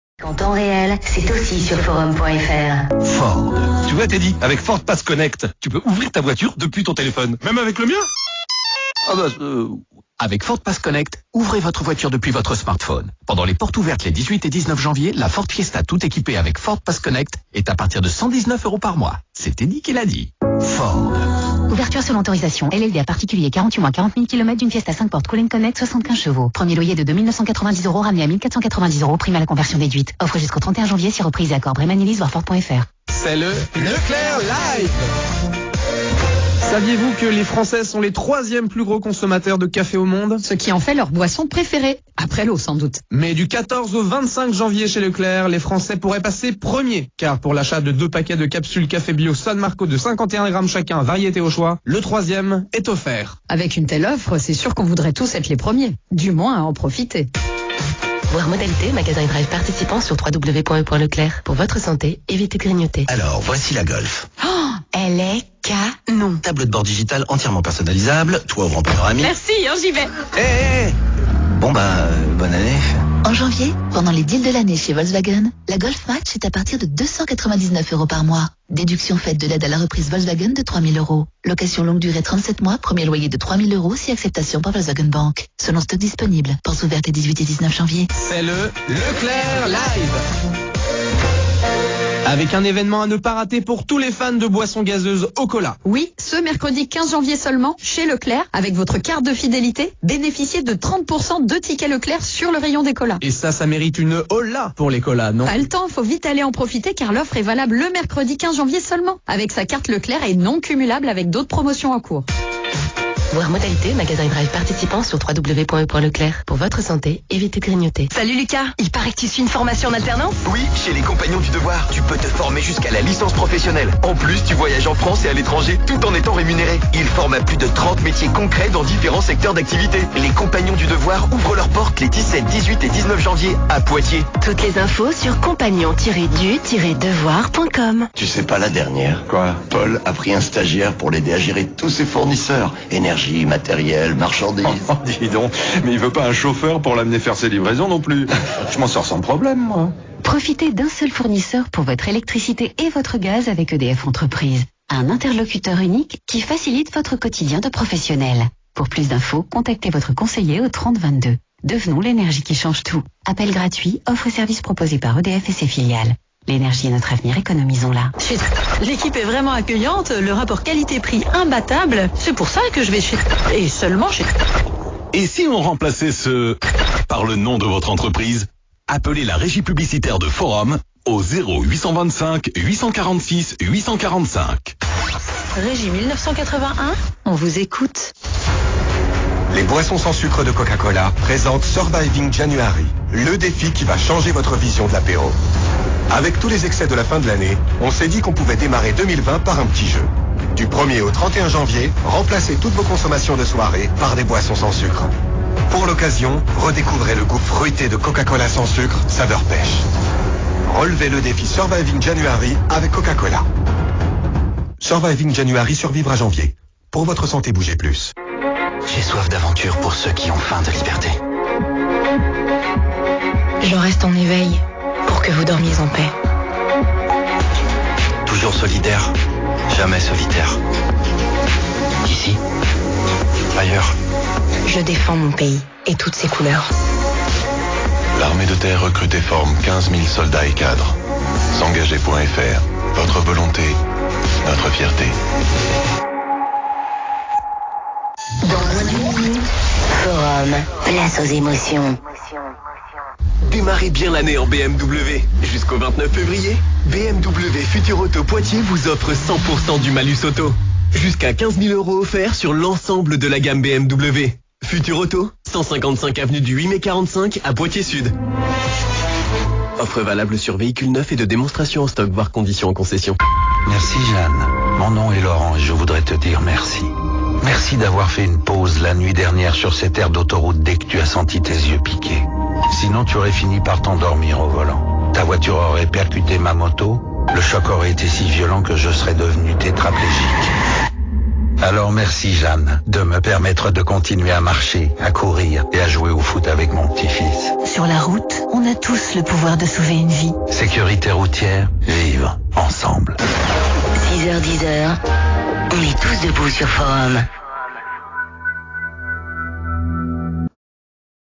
Forum Chauvigny 📻 publicités du 15/01/2020
Forum à Chauvigny 105.2 FM localisé à Chauvigny (86).
Voici les publicités de Forum, le 15/01/2020 à 6:12 du matin.
Liste des pubs :

- Ford
- E.Leclerc
- Volkswagen
- E.Leclerc
- Les Compagnons du Devoir
- EDF
- Régie Publicitaire Forum
- Coca Cola
- S'engager.fr
- BMW
- Sécurité Routière